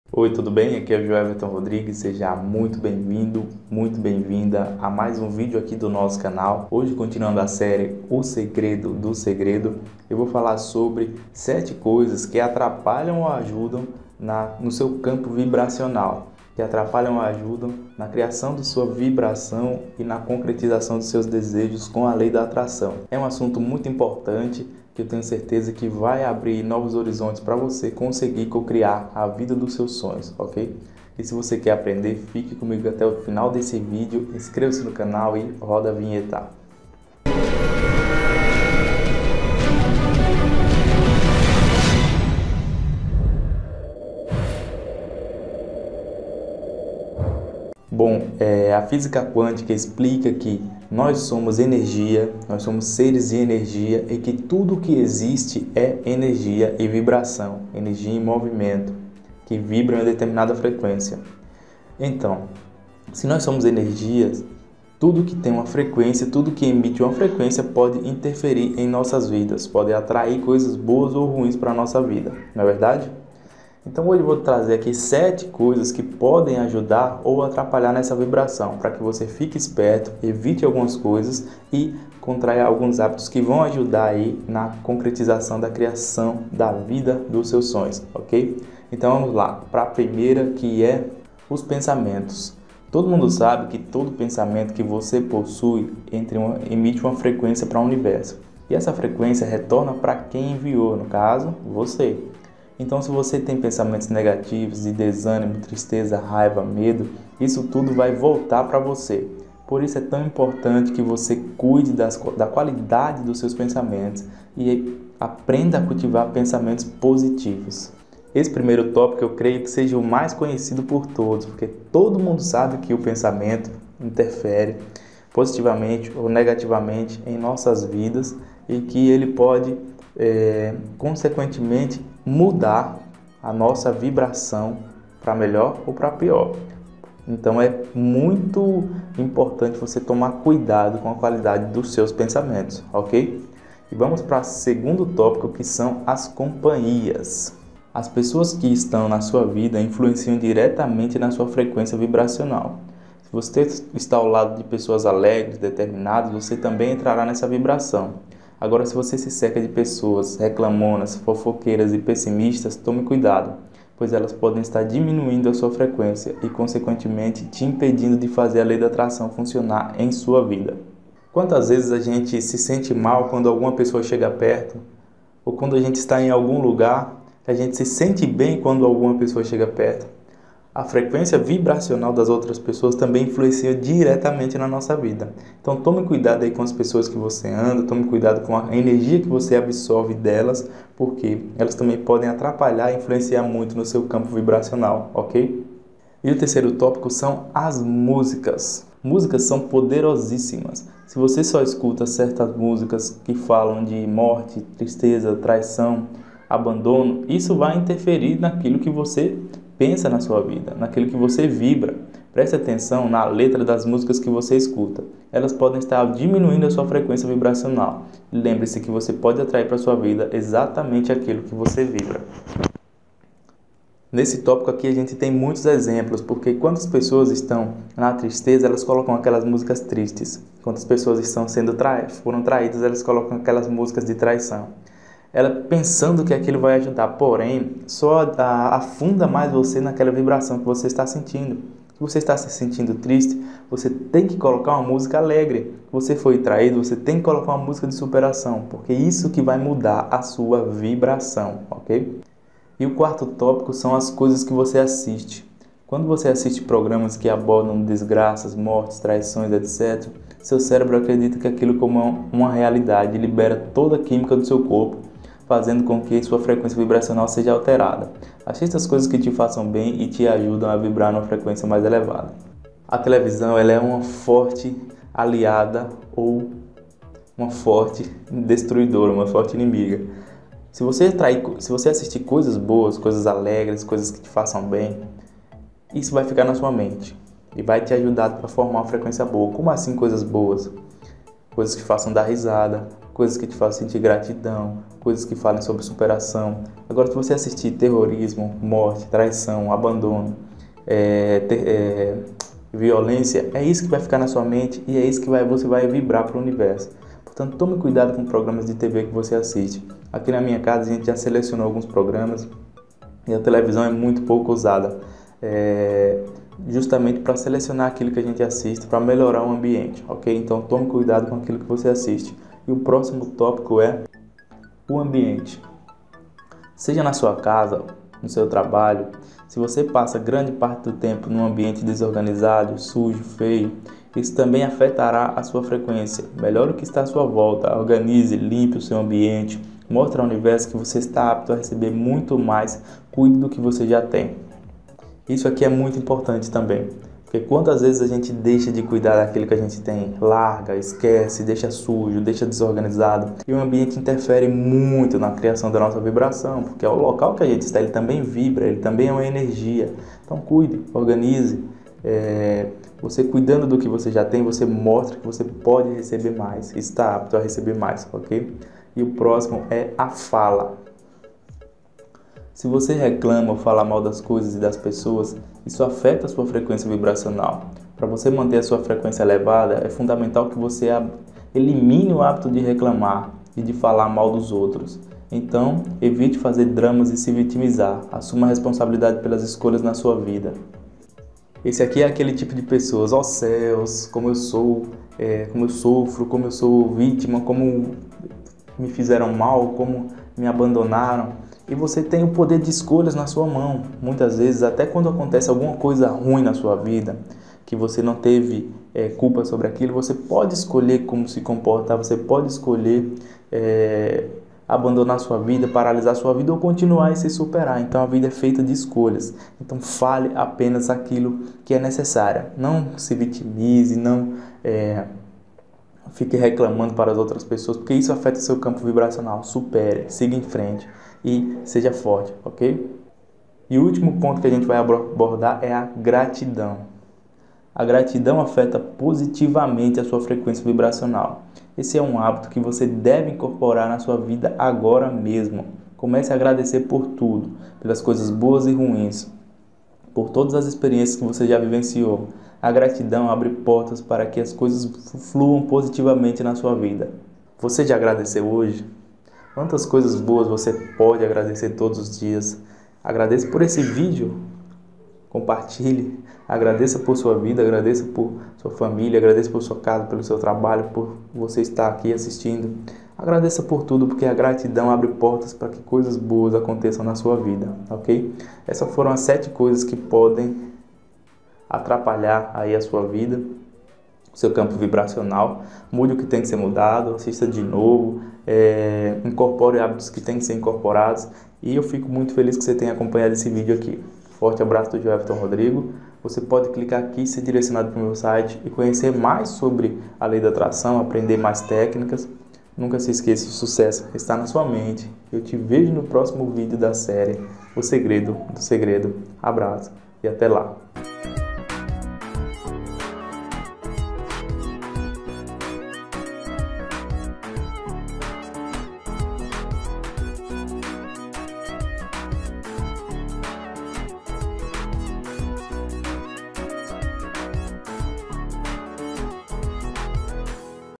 7 Coisas que Alteram SUA VIBRAÇÃO / LEI DA ATRAÇÃO e Física Quântica - O Segredo do Segredo #72
Esse é o  Episódio número 72 da série o segredo do segredo e hoje vou falar SOBRE 7 coisas que Alteram sua Vibração com a Lei da Atração e Física Quântica.

Confira o que pode está te deixando para baixo e alterando sua vibração negativamente, e também descubra como mudar sua vibração positivamente e cocriar com a lei da atração, confira essas dicas de física quântica.